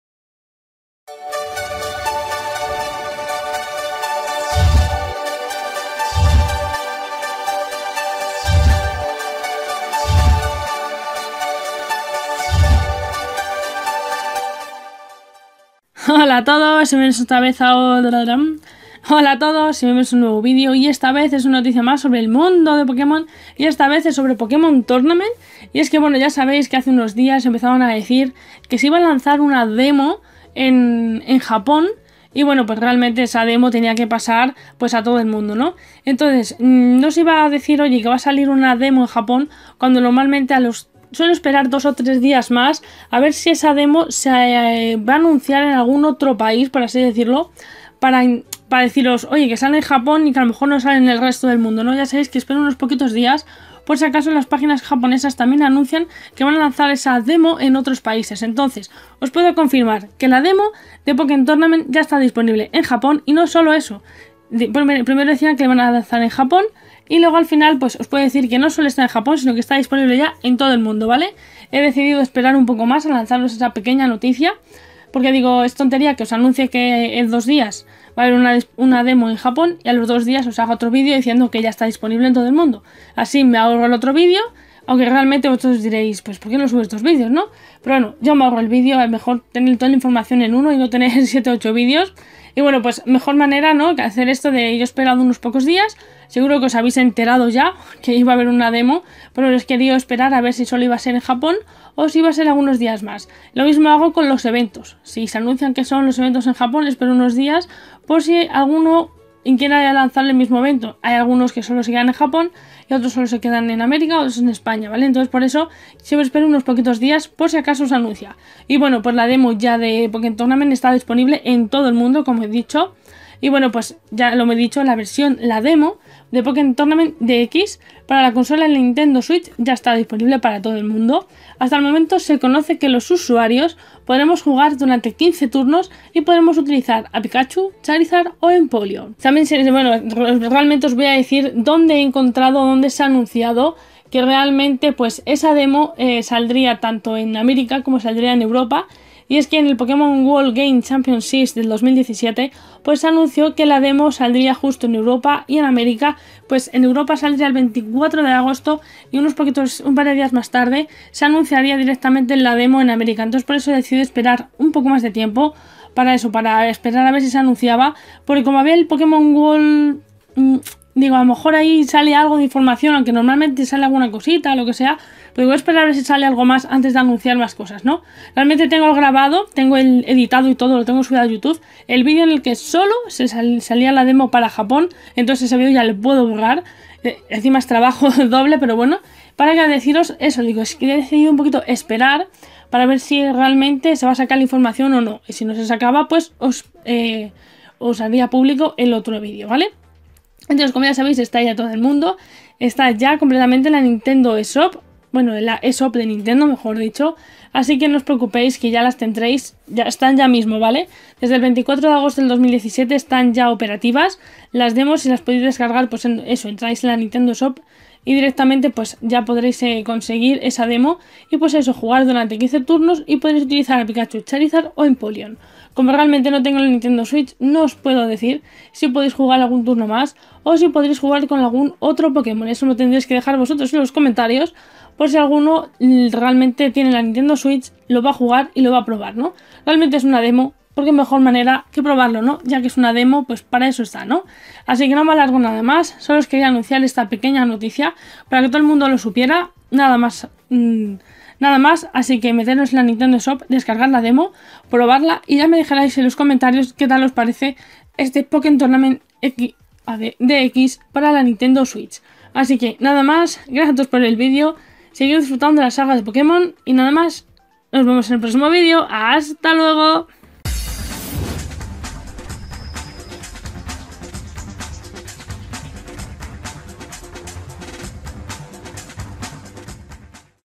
La demo de Pokkén Tournament DX está disponible en todo el mundo
Os cuento un poquito sobre la demo del nuevo juego de Pokémon para la Nintendo Switch, ya disponible para todos.